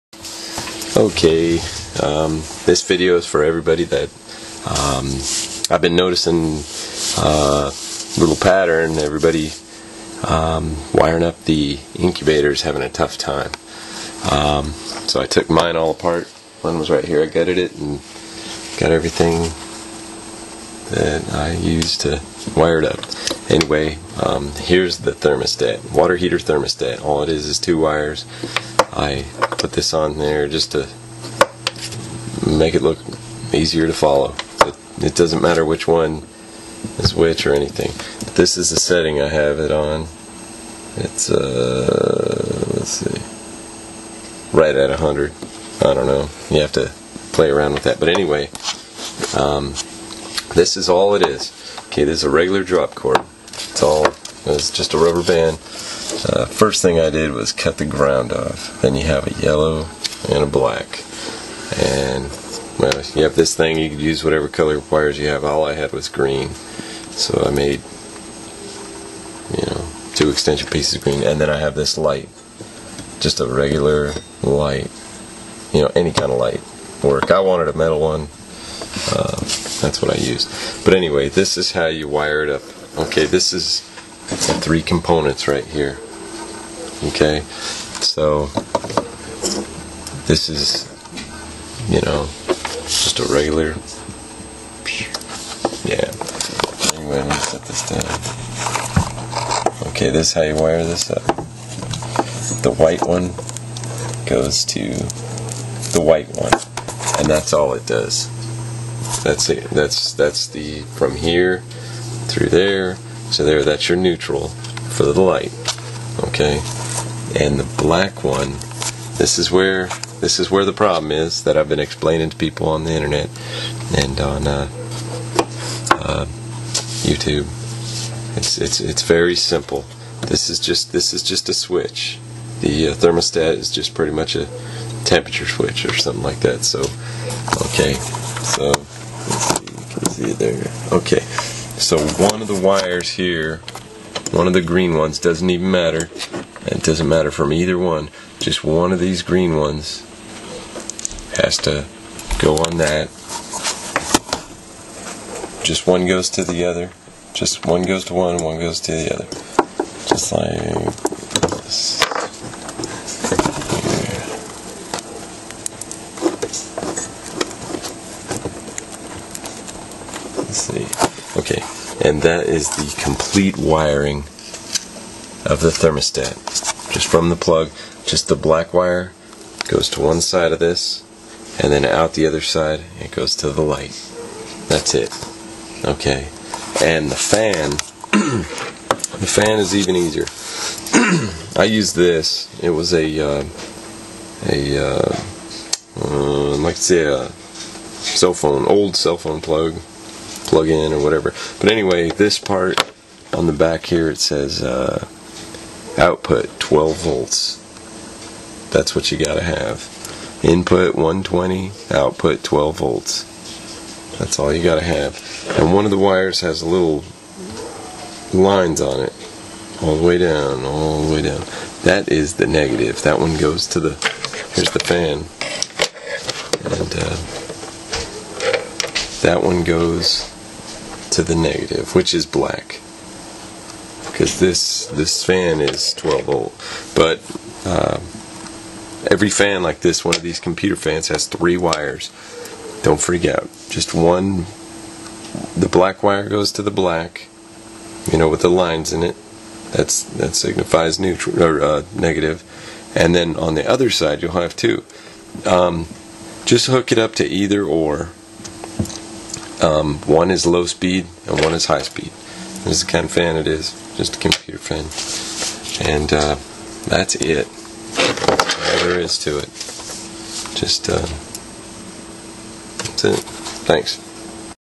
How to wire INCUBATOR for hatching eggs
diy how to wire your home chicken incubator made out of a cooler. this will allow you to wire the light bulb and the computer fan